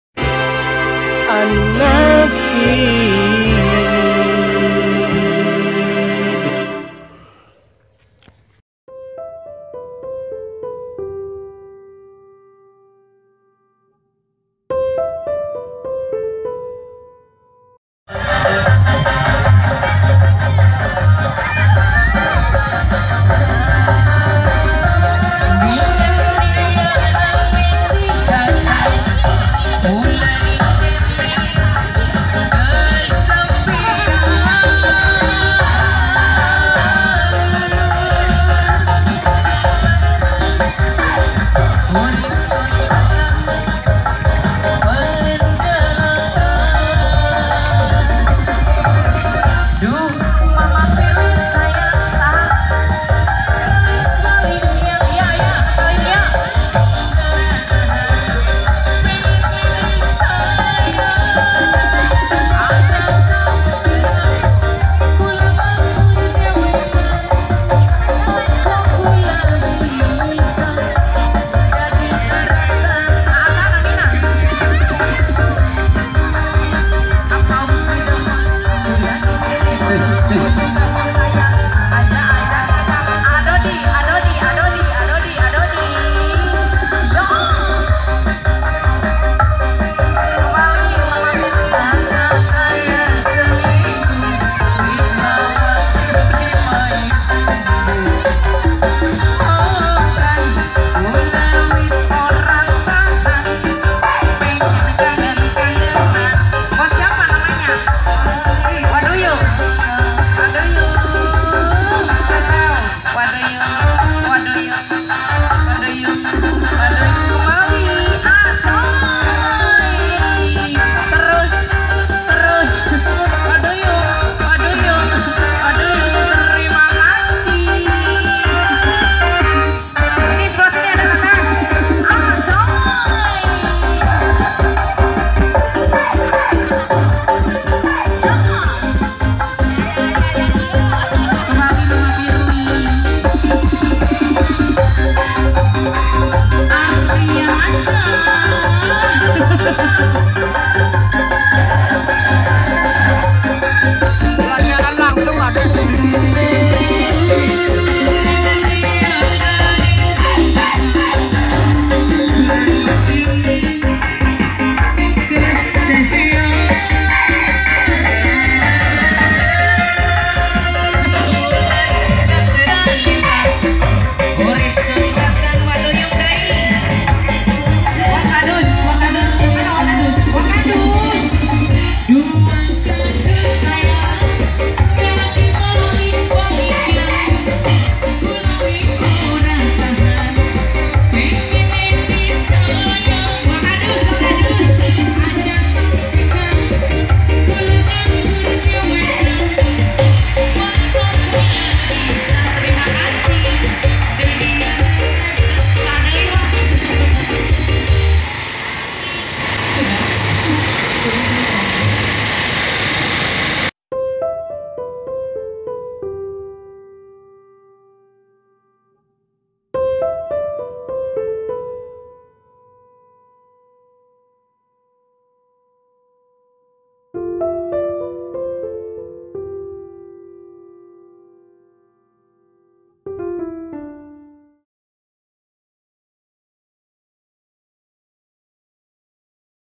#3 SERUNYA GAES II ELAR ELARAN DELAPAN KUDA RENGGONG
DUKUH NANGKA HAJATAN KHITANAN
KUDA RENGGONG  JIMAT, BENTAR, NAZWA, PUTRA ARUMBIA, ABIMANYU, RIZKI, EMAN MUDA, OGIN DAN KUDA PUTRA BUNGSU
VIDEO SHOOTING :
HUB AJA  AURA VIDEOGRAPHY ( CS) HP : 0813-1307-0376
                   AURA PRODUCTION (YOUTUBE)